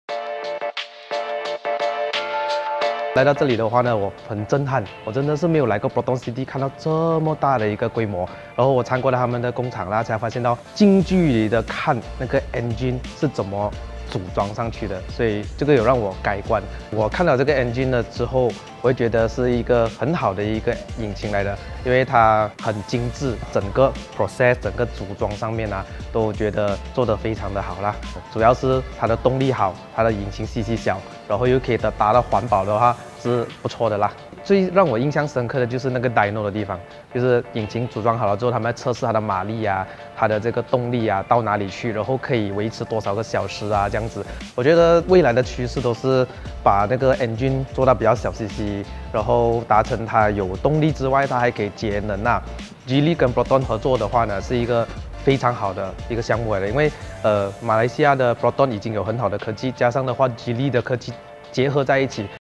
PROTON 1.5 TGDi Engine Factory Visit Media Interview - Aderen Lim 爱的人
An exceptional blend of quality and technology.

Youtuber Aderen, from Aderen Lim 爱的人 visited PROTON manufacturing facility in Tanjung Malim was impressed with the Dyno test and engine assembly process of the PROTON 1.5L TGDi. He believes that this engine offers unbeatable value as it is environmentally friendly while also providing you with maximum power.

What are your thoughts on the PROTON 1.5 TGDi engine?